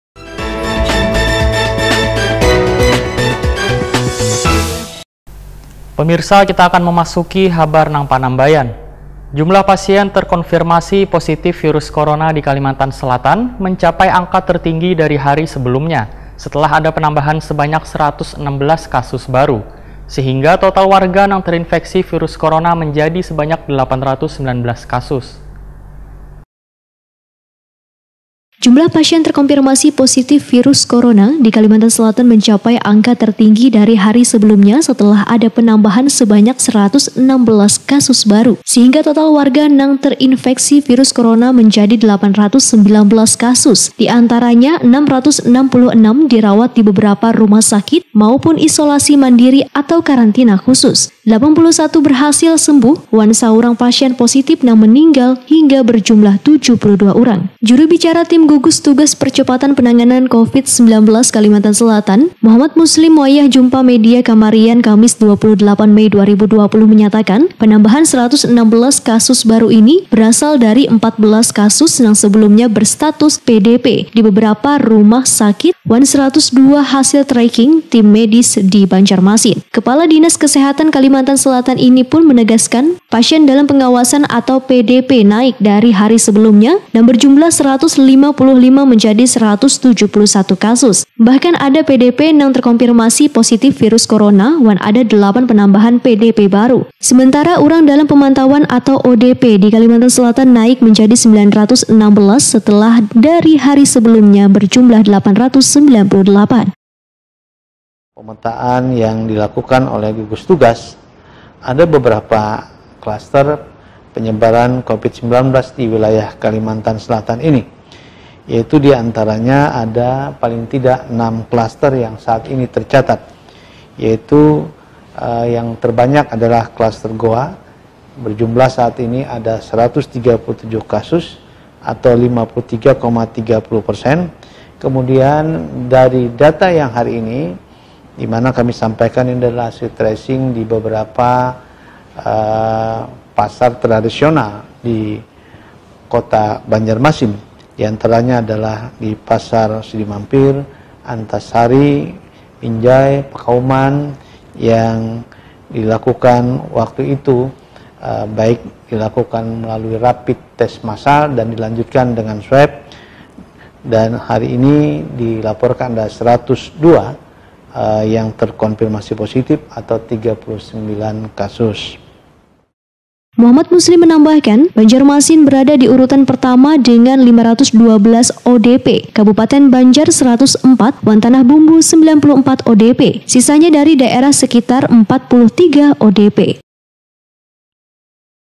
KASUS VIRUS CORONA DI KALSEL TERUS BERTAMBAH
Jumlah pasien terkonfirmasi positif virus corona di Kalimantan Selatan mencapai angka tertinggi dari hari sebelumnya setelah ada panambahan sebanyak 116 kasus baru//  Sehingga/  total warga nang terifeksi virus Corona menjadi 819 kasus/ diantaranya 666 dirawat dibeberapa rumah sakit maupun isolasi mandiri atau karantina khusus/ 81 berhasil sembuh wan saurang pasien positif nang meninggal hingga berjumlah 72 urang//  Juru Bicara Tim Gugus Tugas Percepatan Penanganan Covid19 Kalsel - M Muslim wayah jumpa media/ kamarian Kamis 28 Mei 2020/ menyatakan/ penambahan 116 kasus baru ini berasal dari 14 kasus nang sebelumnya berstatus PDP dibeberapa rumah sakit wan 102 hasil tracing team medis di Banjarmasin//